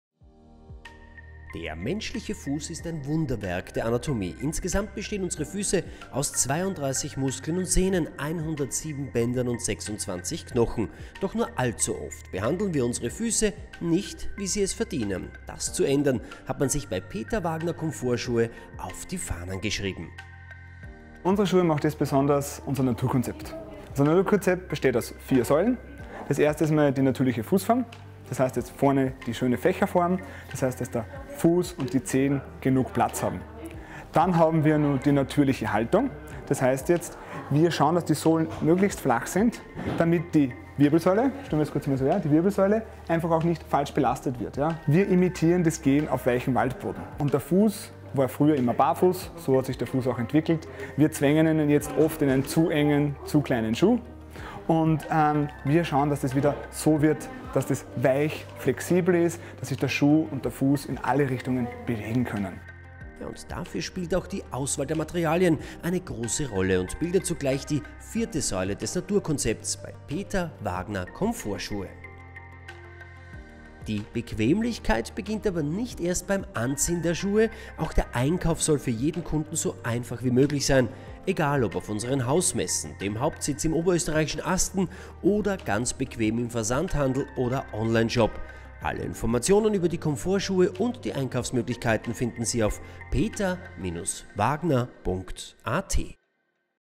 Peter Wagner Comfortschuhe bei Die Meinungsmacher
Erfahren Sie über die Einzigartigkeit der Peter Wagner Comfortschuhe und lernen Sie die Menschen hinter den bequemsten Schuhen Ihres Lebens kennen.